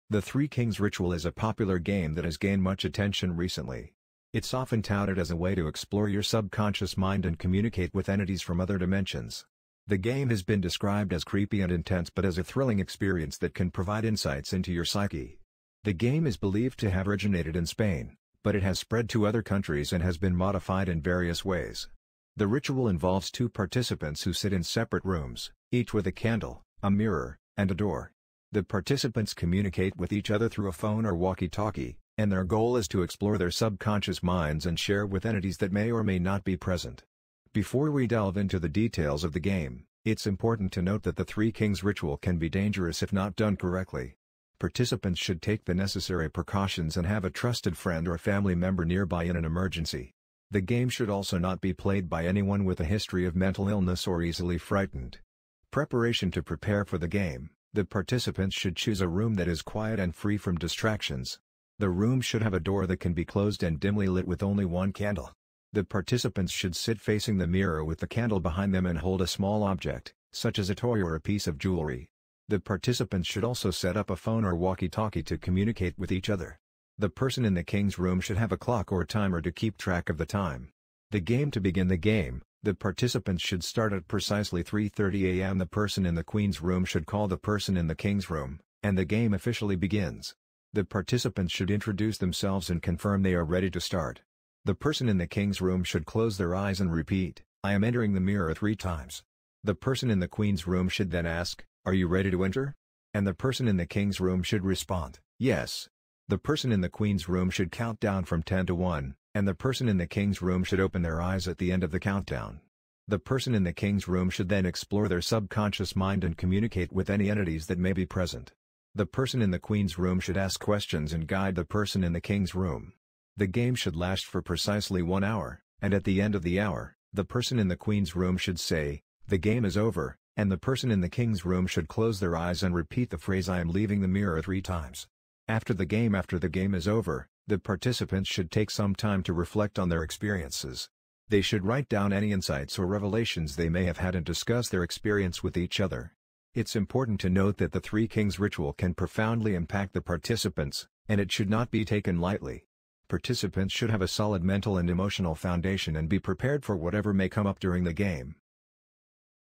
The Terrifying Three Kings Ritual You Must Never Try!
Introduction:
Are you ready to explore the dark and terrifying world of the Three Kings ritual? In this video, we will take you on a journey through the history and practice of this disturbing ritual. Learn about its origins, what it involves, and why it should never be attempted by anyone.

Video Content:
We will dive into the Three Kings ritual, a dangerous game that is said to open a portal to the spirit world. This ritual is not for the faint of heart and should never be taken lightly. We will explore the steps involved, the necessary tools, and the potential consequences of playing the game.

In addition, we will share some true stories of people who have tried the Three Kings ritual, and the horrifying results that followed. From unexpected visitors to ghostly sightings, these experiences will send shivers down your spine.

Overview:
The Three Kings ritual is a dangerous game that should never be attempted. The ritual involves creating a specific set-up in your room, using mirrors, candles, and a chair, to open a portal to the spirit world. Once the portal is open, players will have the opportunity to interact with spirits and explore the unknown. However, this game comes with serious risks, including mental and physical harm, and is not for the faint of heart.